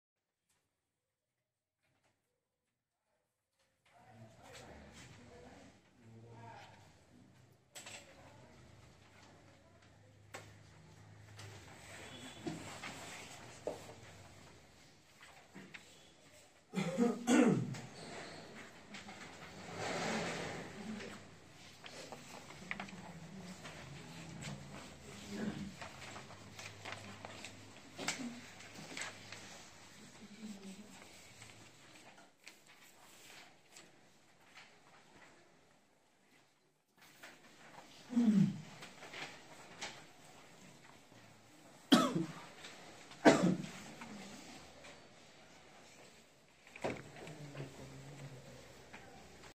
चाणक्य एकेडेमीको देनिक MOCK Test को एक झलक ।
चाणक्य एकेडेमीमा सञ्चालित लोकसेवा तयारी कक्षाको एक झलक । आउनुहोस हामी तपाईहरुको नेपाल प्रहरी देखि विभिन्न राष्ट्रसेवक बन्ने यात्राको तयारीमा सहयोगी भूमिका निभाउछौ , कक्षामा सहभागी बनौ ।